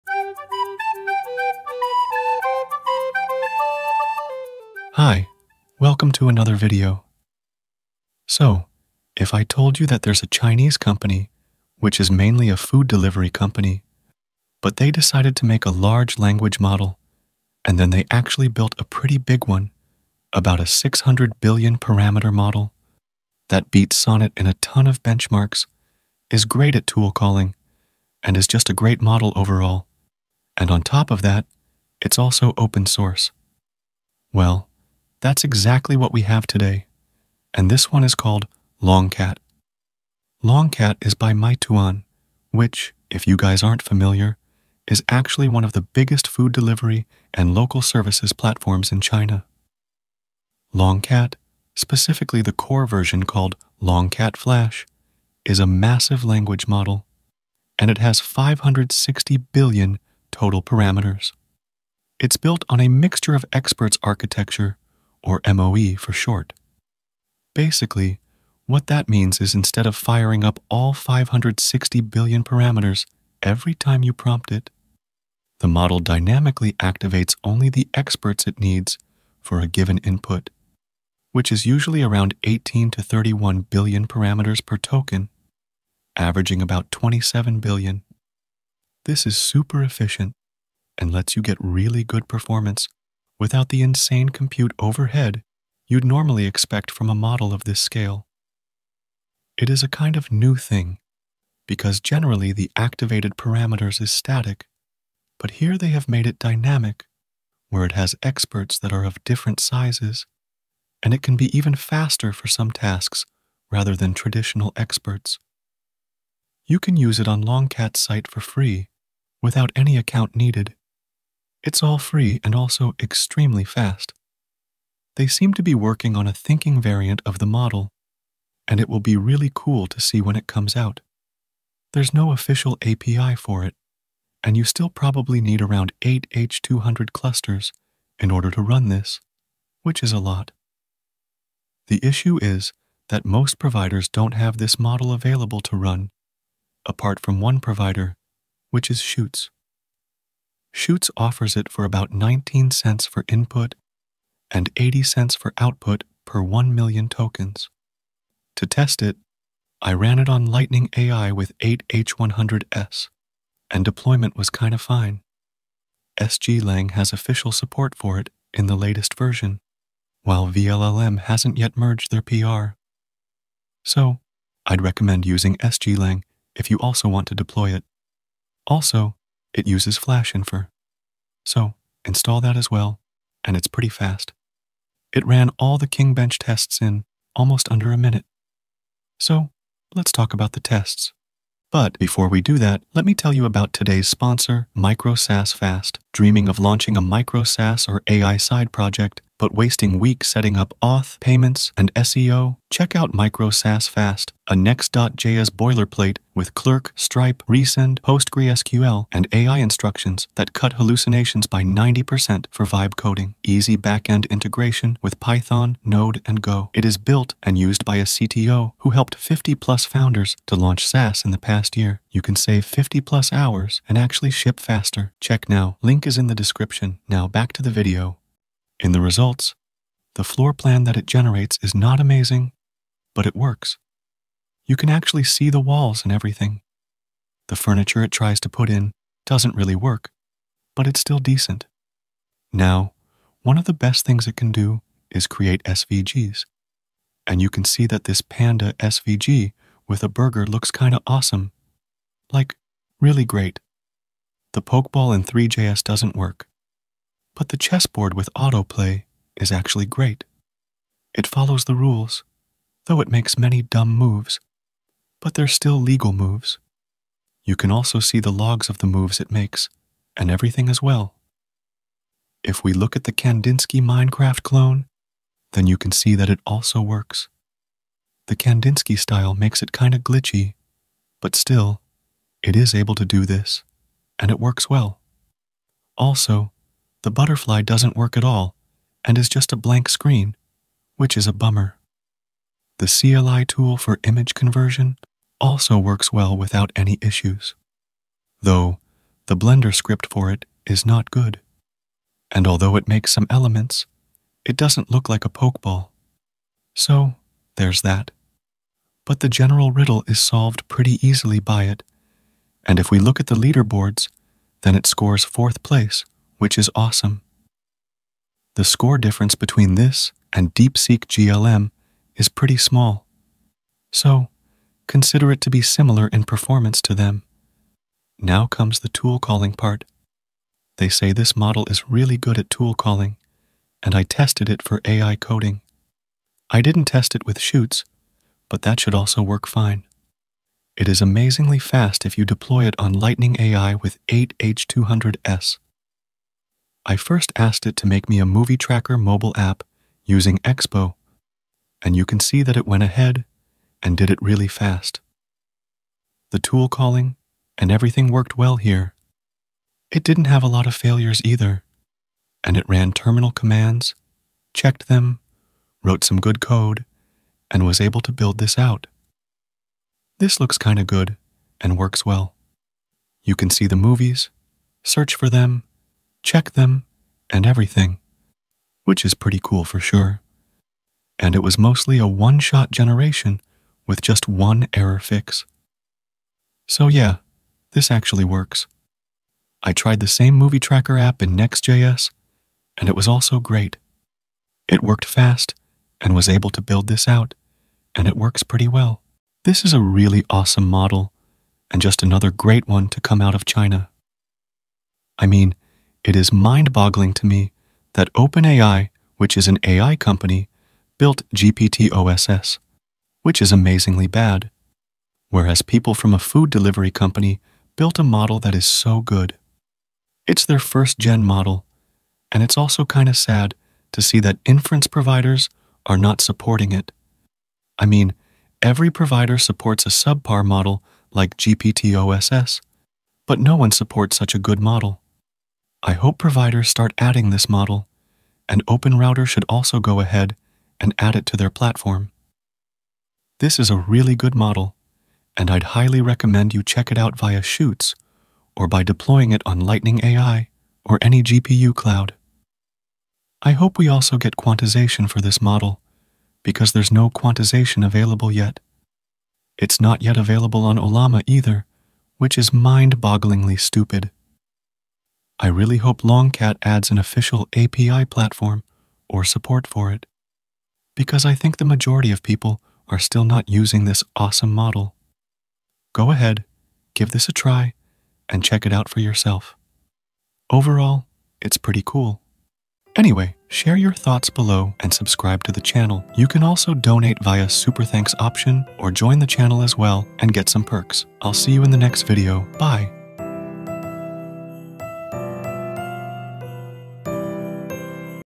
LongCat (560B): Bye Sonnet! Why is NO ONE Talking about this!? This OPEN Model is AWESOME!
In this video, I’ll be telling you about LongCat, a brand-new open-source large language model from Meituan, a Chinese food delivery giant, that surprisingly built a 560B parameter MoE model beating Sonnet in benchmarks, excelling at tool calling, and running super efficiently.

Key Takeaways:

🚀 LongCat is a 560B parameter Mixture-of-Experts model built by Meituan, one of China’s biggest food delivery companies.
⚡ It activates only 18–31B parameters per token, making it highly efficient and extremely fast.
🌐 You can try it for free on LongCat’s site, though official APIs are not yet available.
💸 Chutes offers paid access at $0.19 input / $0.80 output per 1M tokens.
🛠️ Works with SG-Lang and FlashInfer, with Lightning AI deployment tested on 8 H100/H200 clusters.
📊 Performs well on KingBench, solving riddles, generating SVGs, and even building apps with tool calling.
🎮 Some outputs like Pokeball in ThreeJS and butterfly rendering fail, but chess, Minecraft clone, and CLI tools work great.
🤖 Outperforms GPT-OSS in many areas, showing how strong China’s first-gen models are.
🔗 Still lacks quantization, Ollama support, and official APIs, but has huge potential if providers adopt it.
👍 Overall, LongCat is an impressive open-source model worth trying out.

Timestamps:

00:00 - Introduction
00:07 - What is LongCat?
01:56 - How to Use LongCat
03:11 - MicroSaaSFast (Sponsor)
03:48 - KingBench Test Results
05:38 - Tool Calling & AI Coding Tests
08:02 - Ending